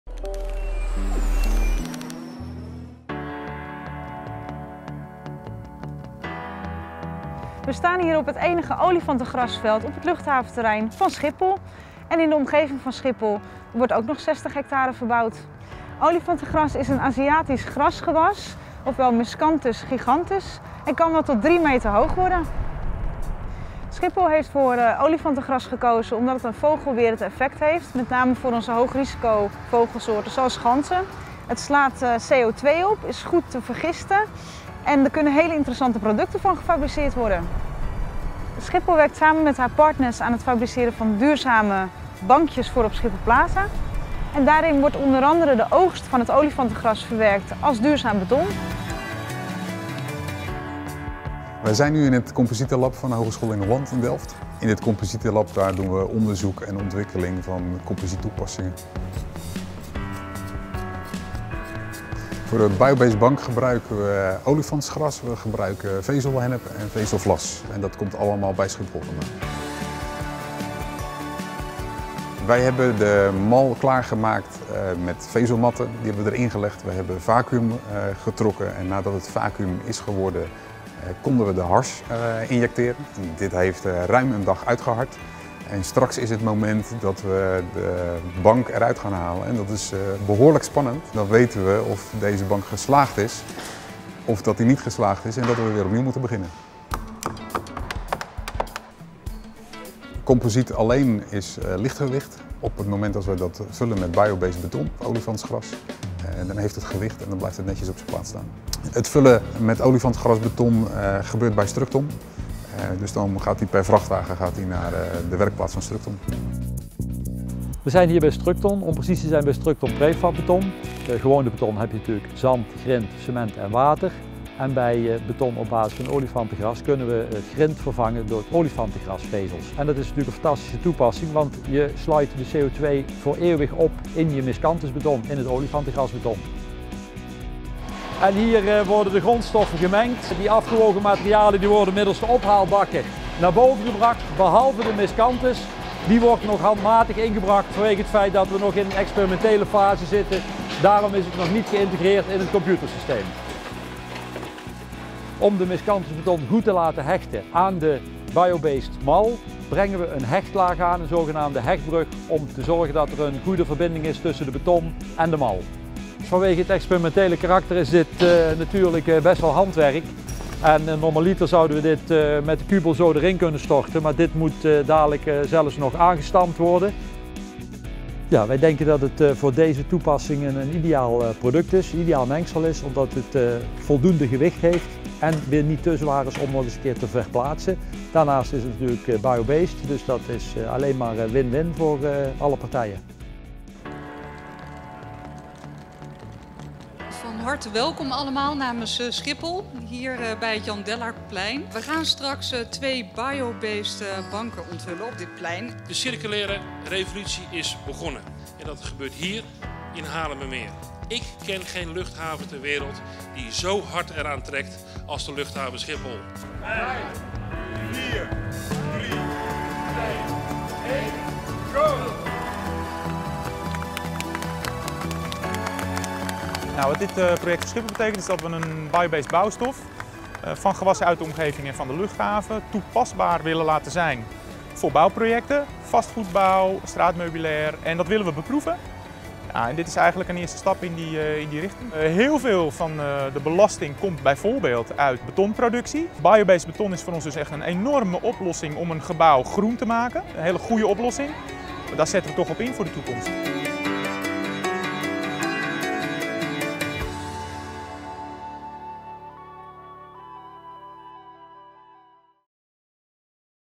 Duurzame bankjes van olifantengras op Schiphol
Van olifantengras, dat groeit op en rond Schiphol, laten we duurzame bankjes maken. Heb jij ze al gespot op het plein voor Schiphol Plaza?!